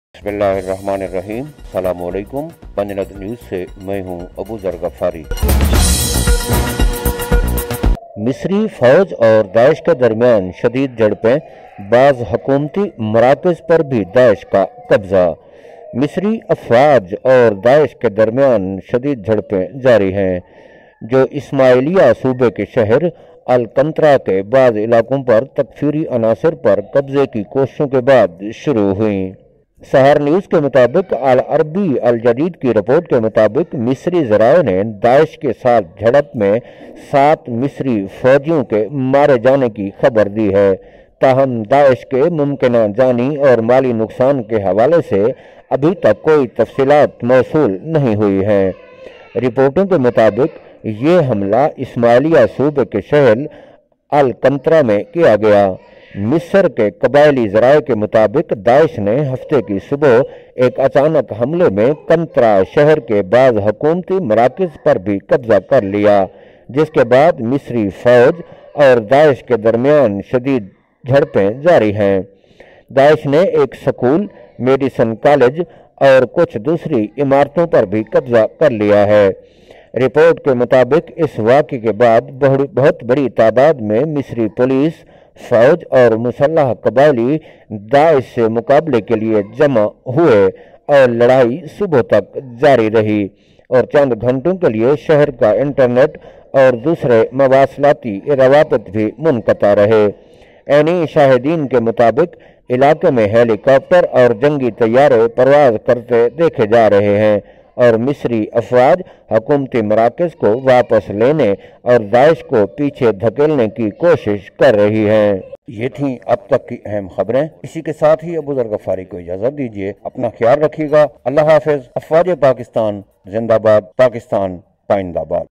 Fierce clashes between the Egyptian army and Daesh, some government centers occupied by Daesh
مصری فوج اور داعش کے درمیان شدید جھڑپیں، بعض حکومتی مراکز پر داعش کا قبضہ
Fierce clashes are ongoing between the Egyptian forces and Daesh, which began after Takfiri elements attempted to capture some areas of the city of Al-Qantara in Ismailia province
According to the report of Al-Arabi Al-Jadeed, Egyptian sources have reported that seven Egyptian soldiers were killed in the clash with Daesh, however, details regarding the possible loss of life and property of Daesh have not yet been received.

According to the report, this attack was carried out in the city of Al-Qantara in Ismailia province. According to Egyptian tribal sources, Daesh captured some government centers in the city of Qantara in a surprise attack on Saturday morning, after which fierce clashes began between the Egyptian army and Daesh. ISIS has captured a school, medicine college and some other buildings.

According to the report, after this incident, a large number of Egyptian police, army and armed tribesmen gathered to fight ISIS and the fighting continued until morning and the city's internet and other communication links were cut off for a few hours.

According to eyewitnesses, helicopters and warplanes are seen flying over the area and Egyptian forces are trying to retake government centers and push back ISIS

Panjnad News